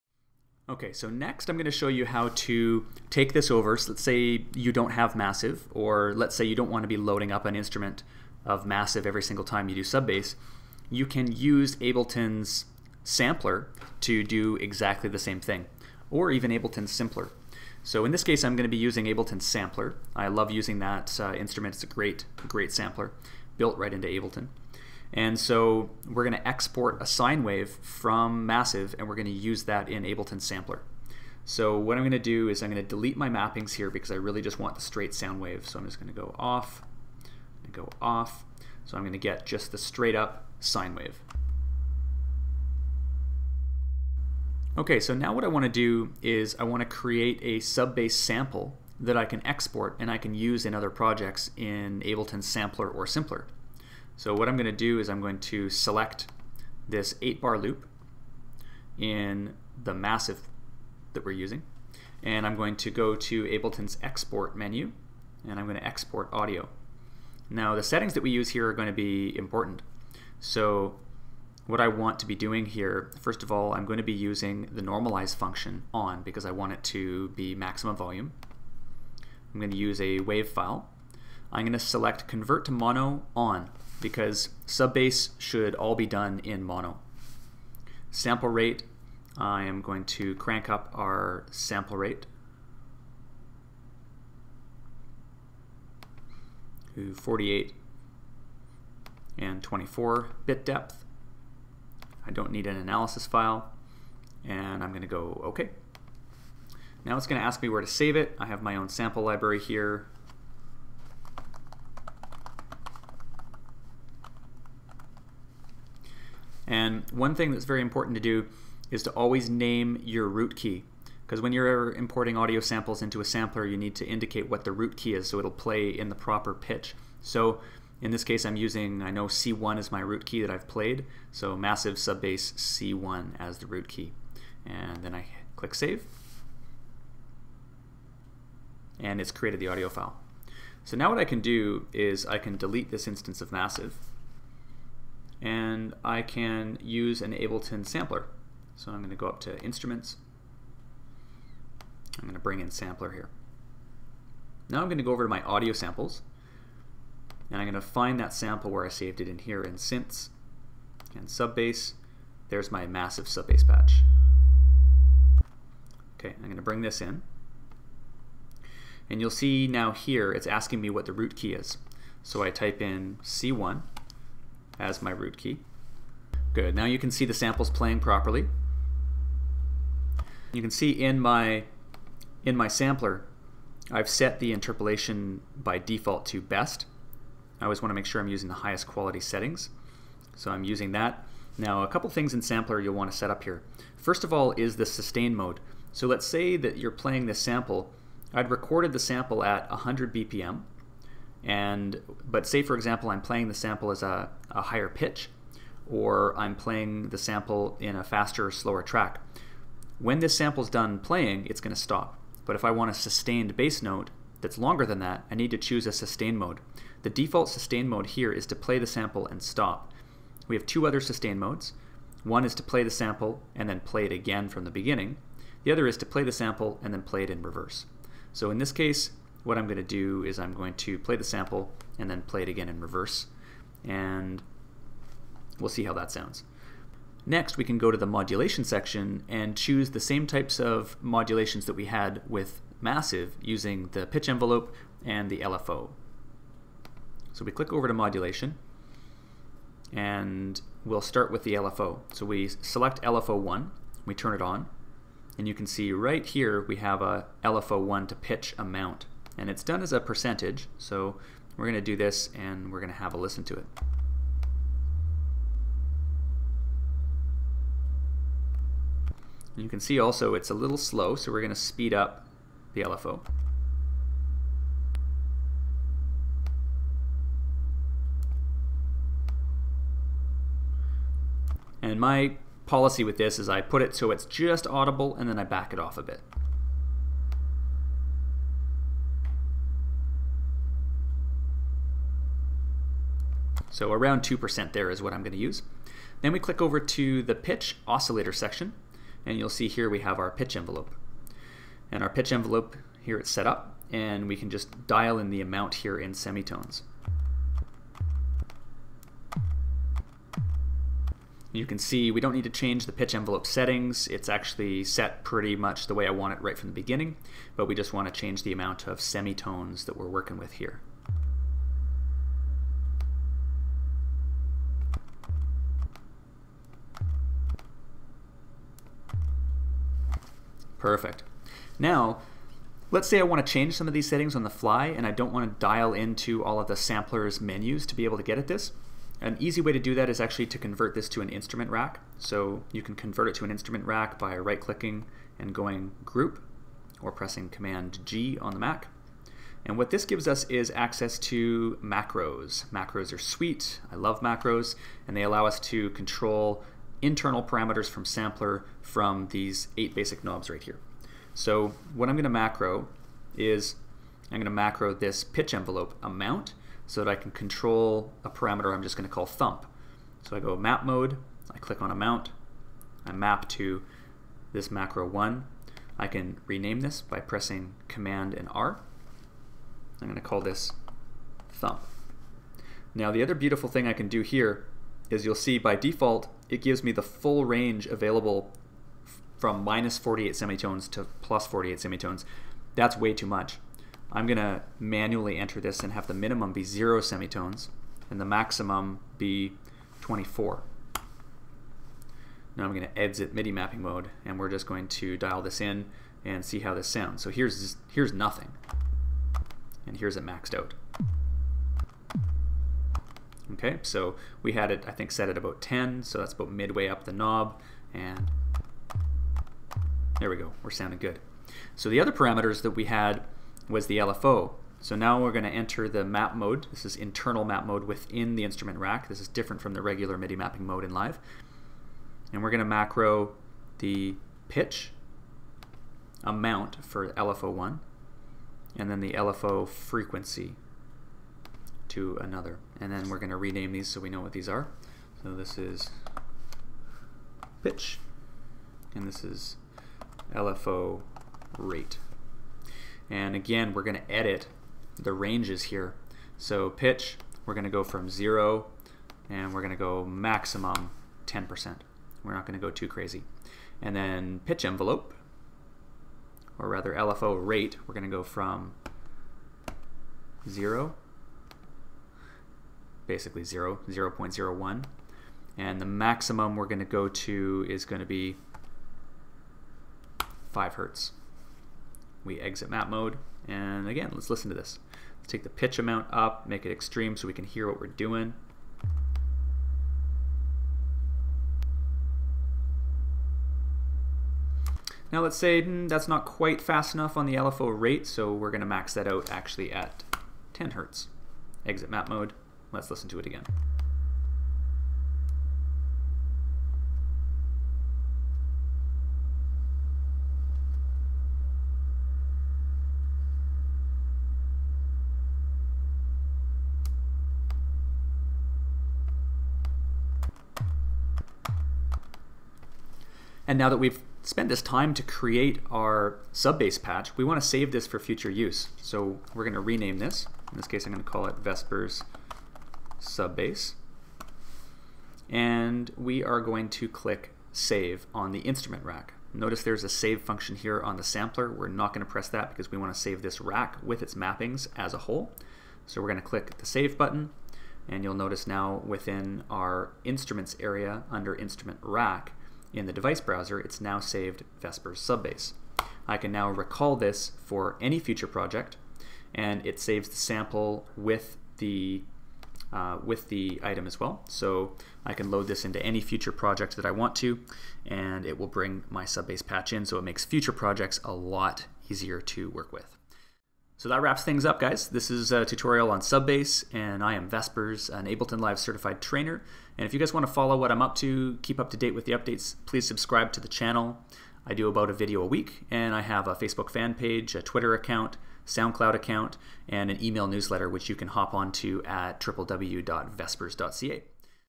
SUB BASS Secrets in Ableton SAMPLER Pt 2 of 2
Ableton Live Certified Trainer HD tutorial: Learn the secrets for making thundering sub bass in Ableton Sampler.